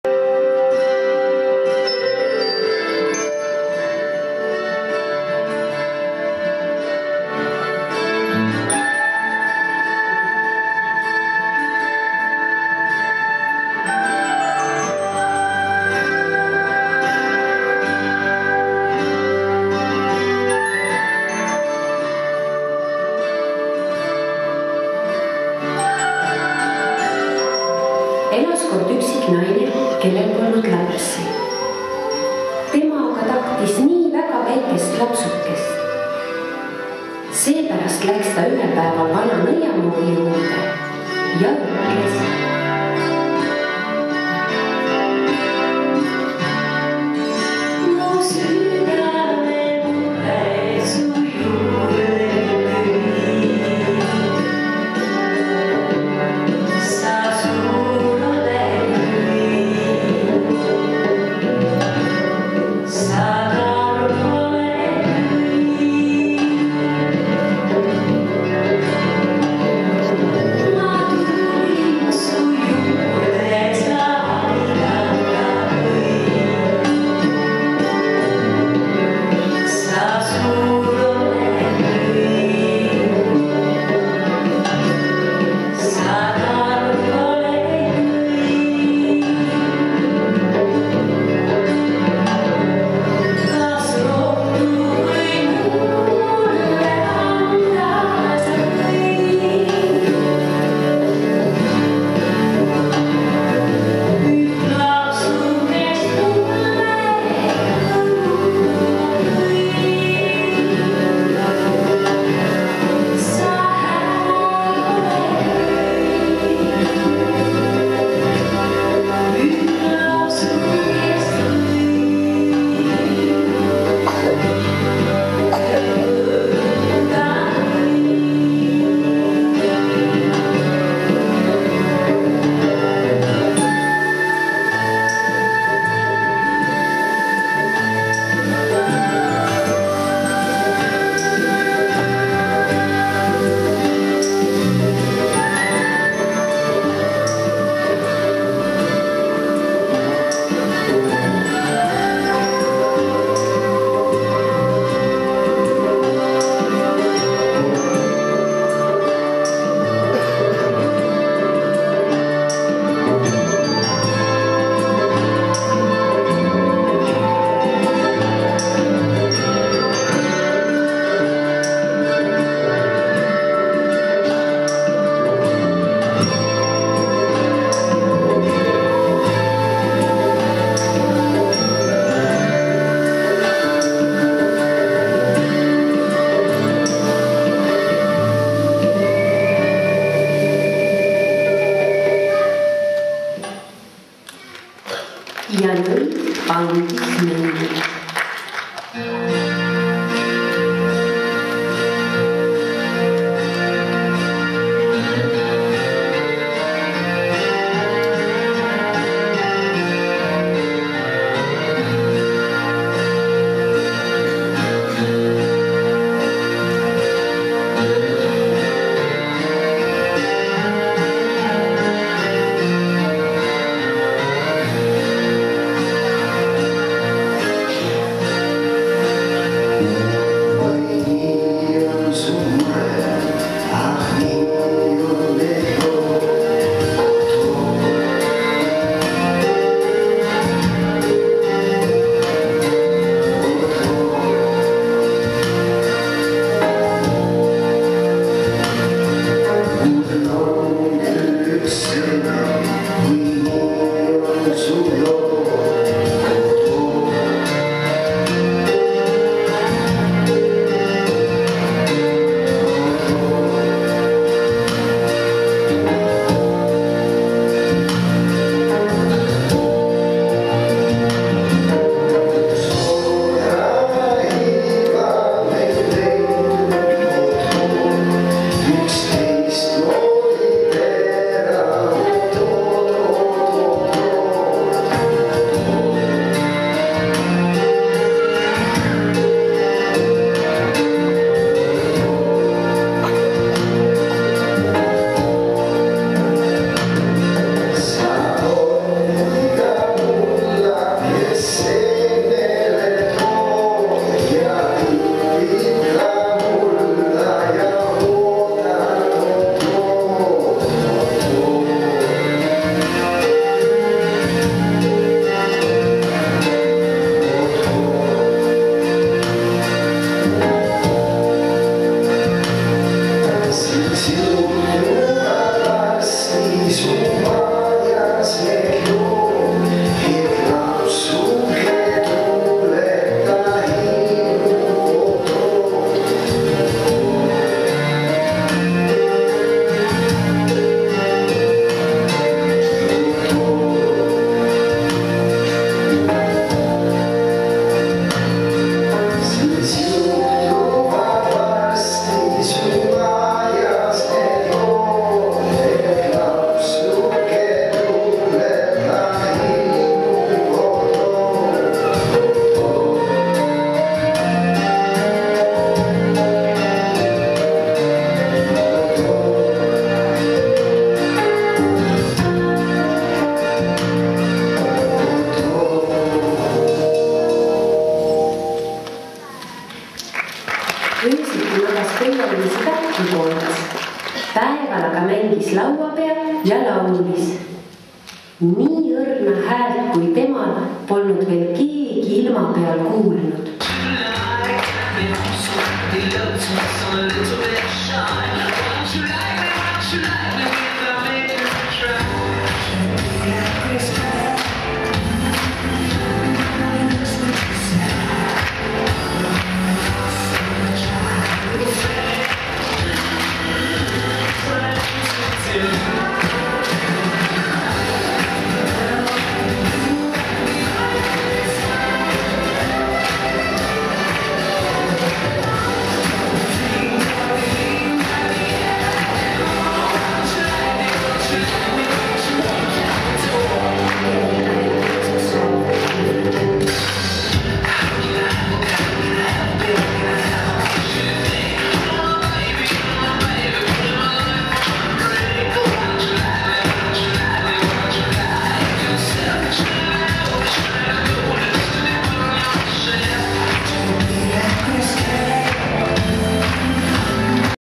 Pöial-Liisi algustants - Laine Mägi Tantsukool
Laine Mägi Tantsukooli jõulukontsert 2008. Laine Mägi Dance School Christmas Concert 2008.